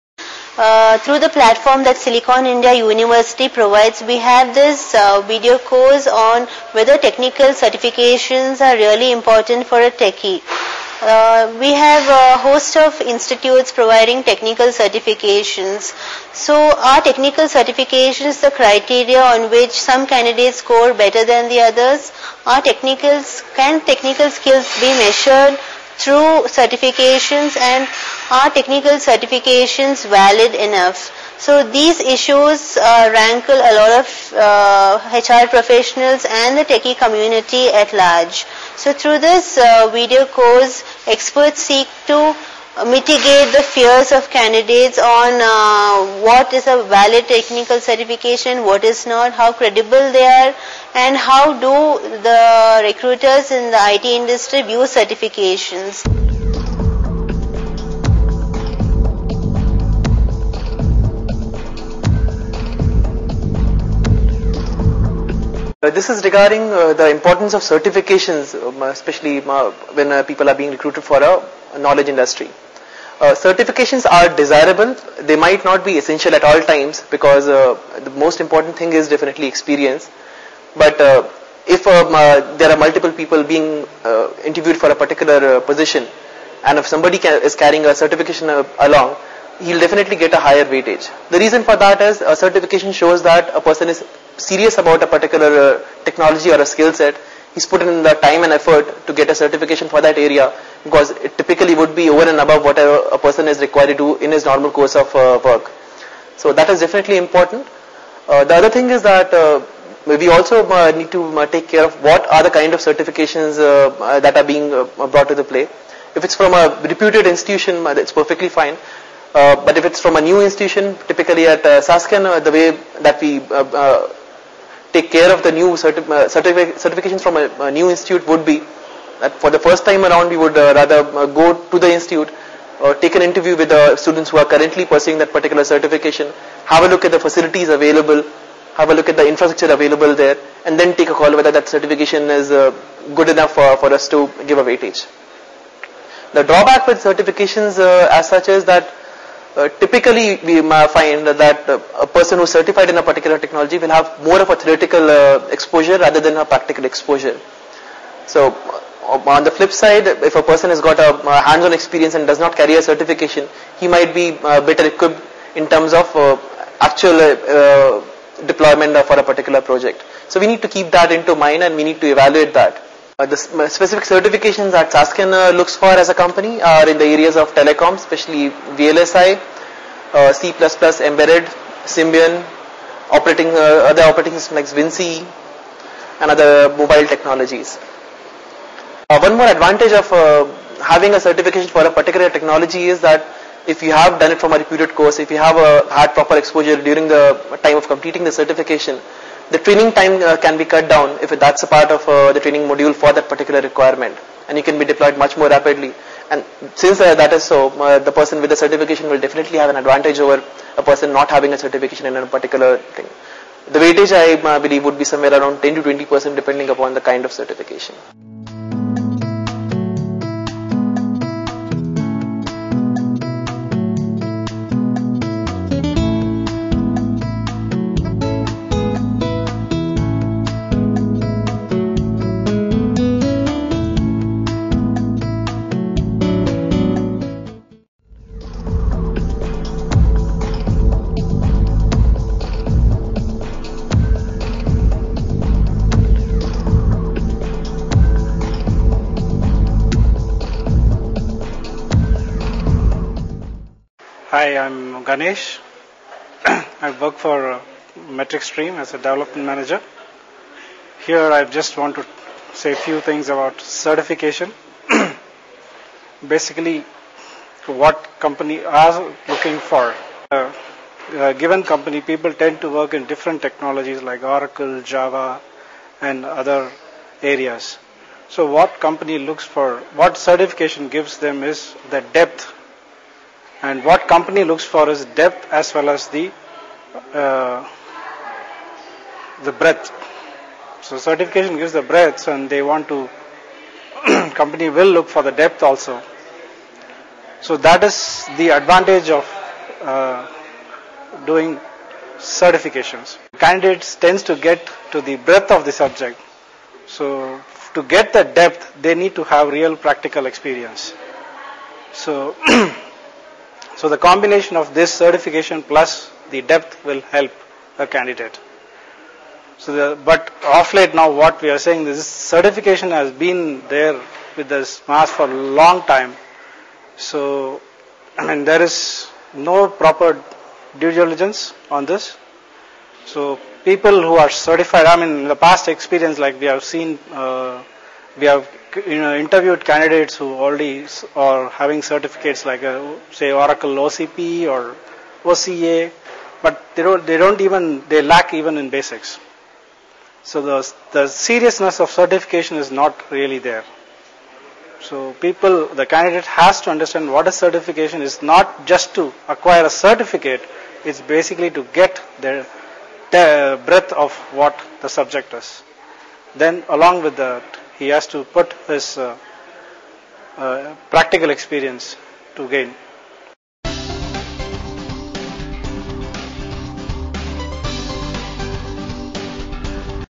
How much value do certifications add to a skill set of an ex
It is often a notion that certified professionals score better in an interview than those who are not certified. Are certifications really the main criteria for judging the technical skills of an IT professional? How much value do certifications add to your resume?

In this video course, experts from the industry voice their opinion on the necessity of certifications for techies.